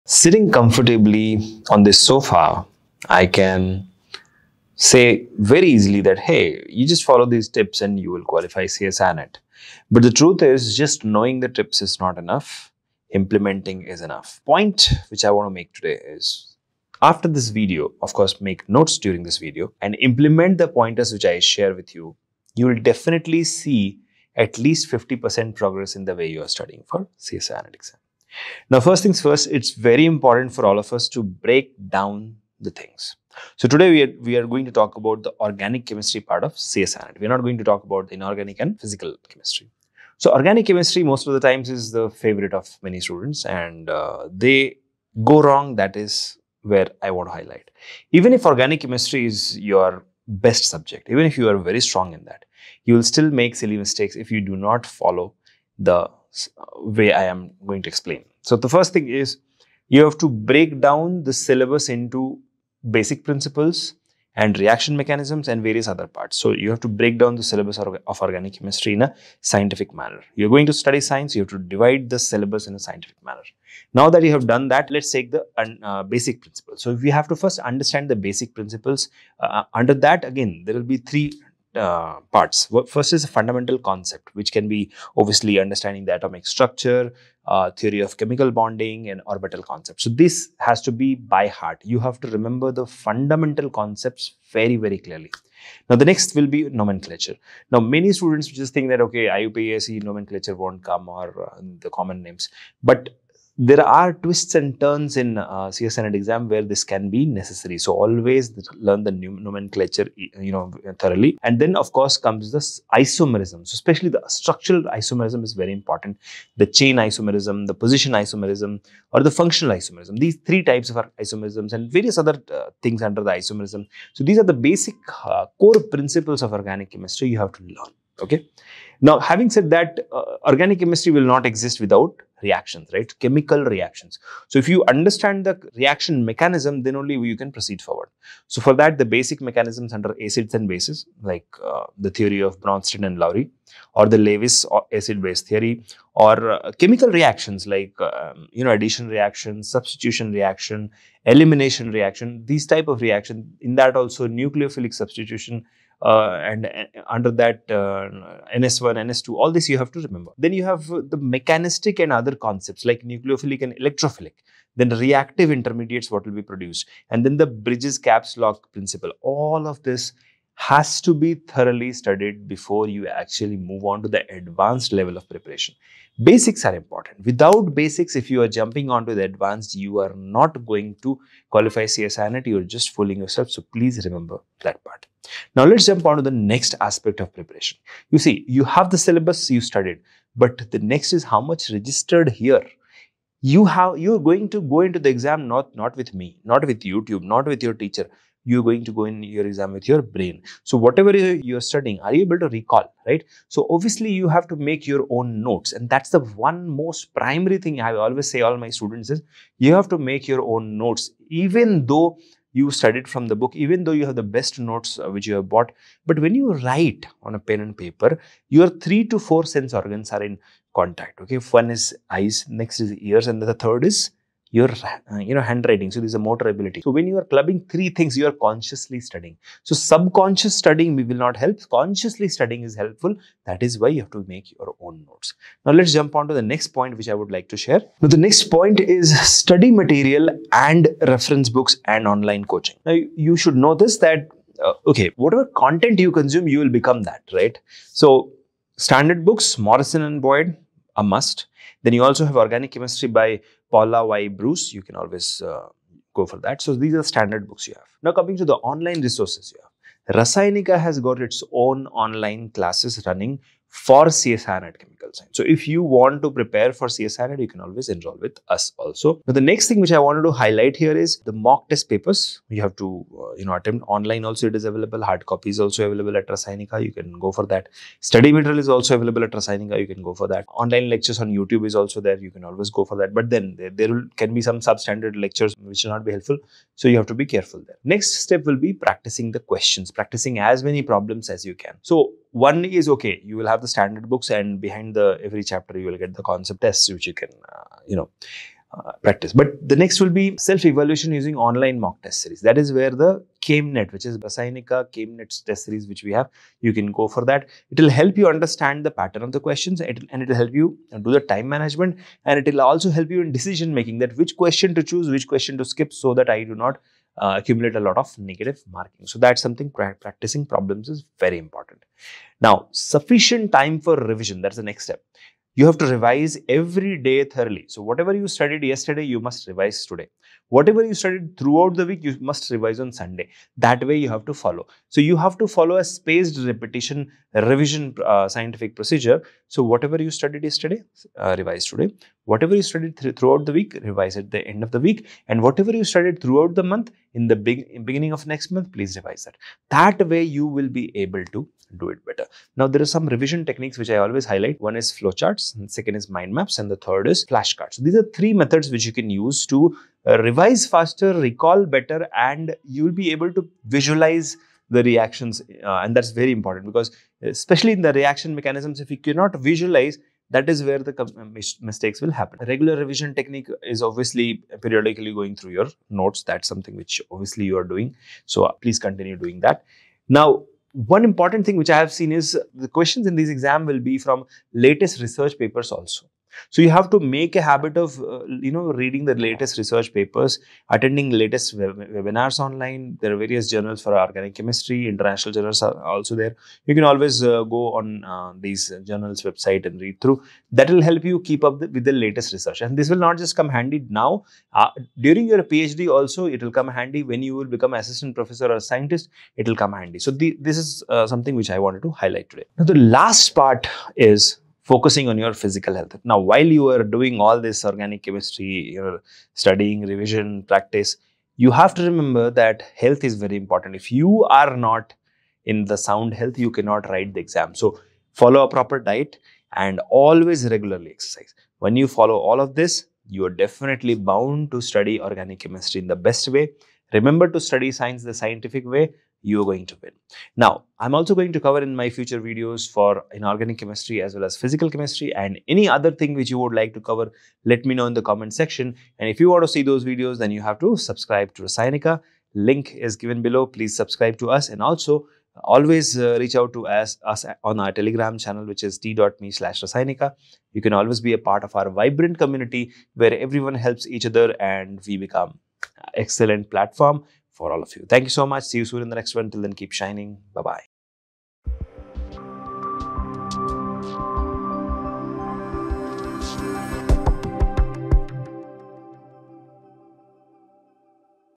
Tips To Study Organic Chemistry For CSIR NET Exams - Must Watch Video
If you're preparing for the CSIR NET Exams and Organic Chemistry is posing a challenge, this video is a must-watch. We've condensed valuable tips and strategies to help you conquer Organic Chemistry and excel in your exams. Our expert advice, tailored to the CSIR NET syllabus, covers study techniques, recommended resources, and problem-solving approaches. Whether you're a newcomer or looking to fine-tune your knowledge, this video is packed with insights to boost your confidence.